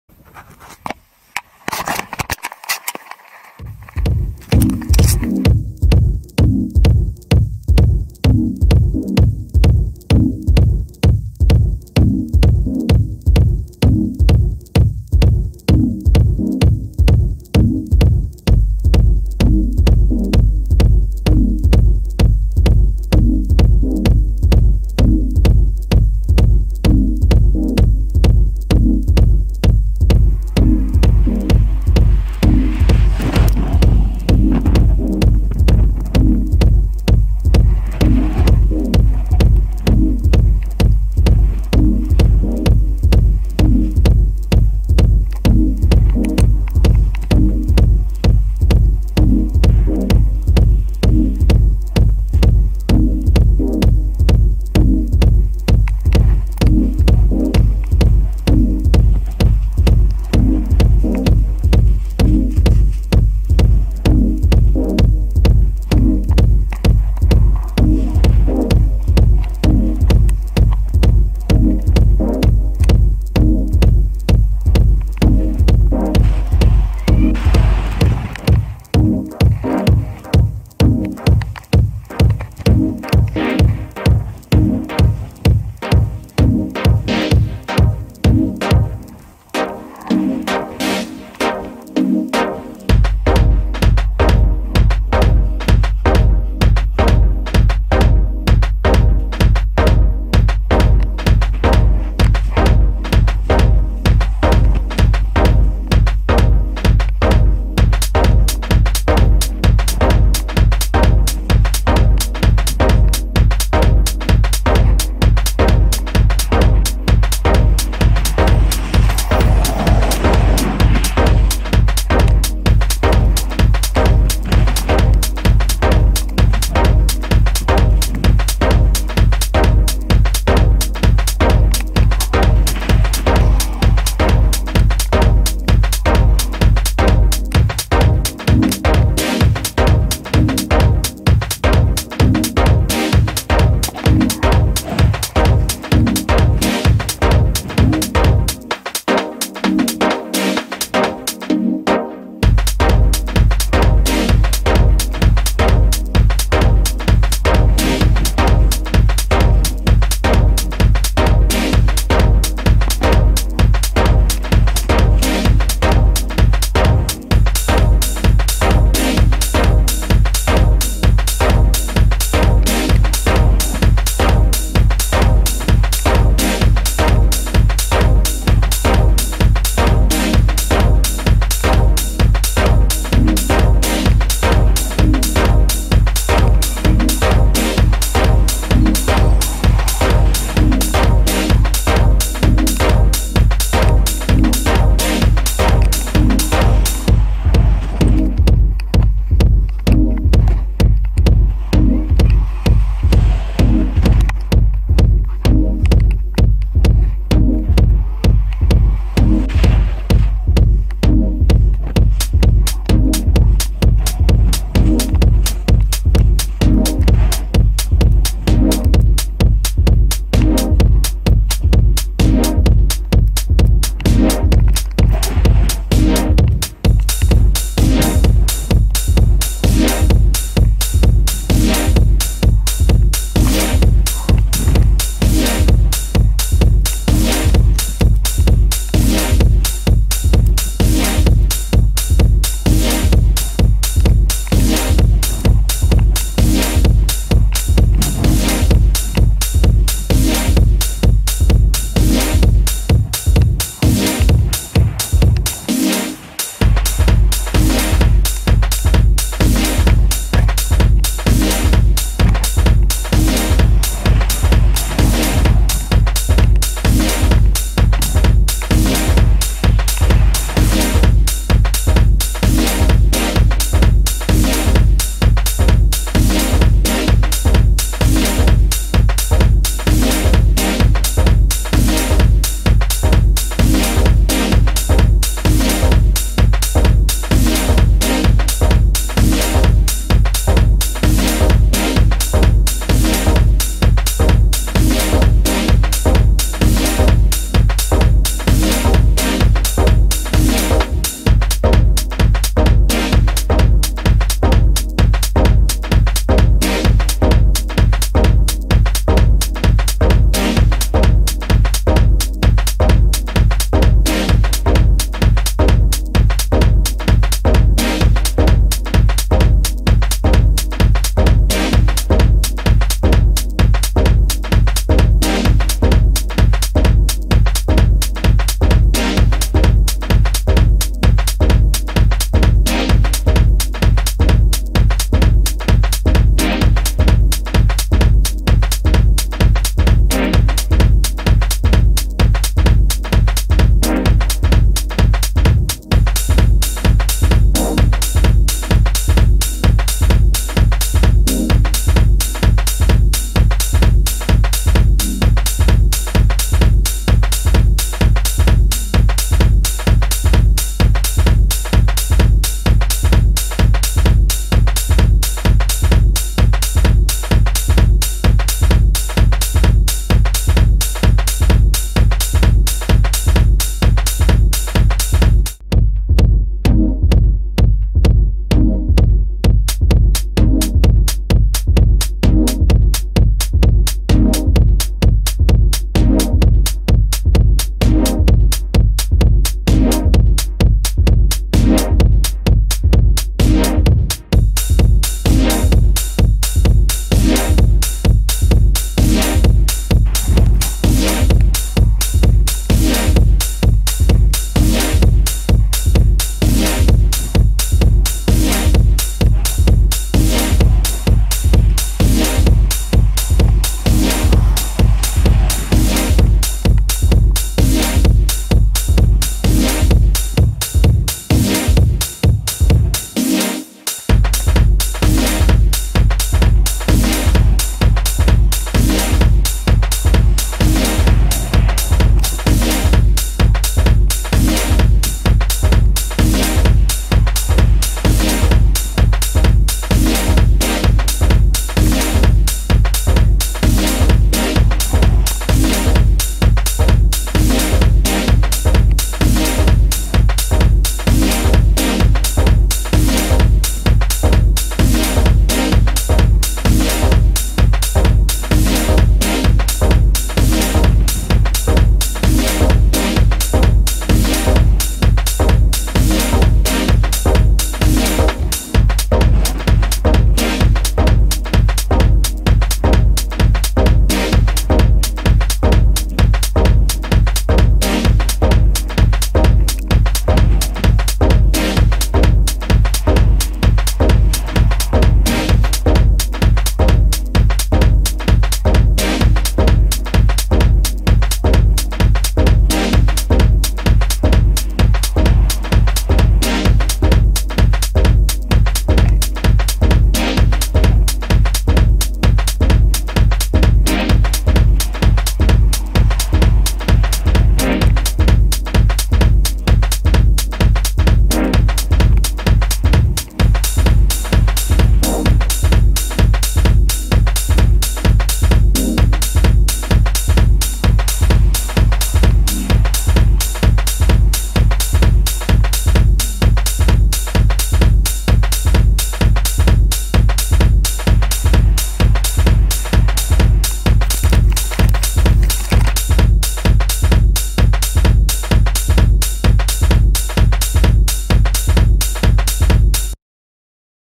Trippy flights | 飛んでみな。飛ぶぞ。
Misty Flip, Mac Twist and Helico training.
My swim vest tend to be open.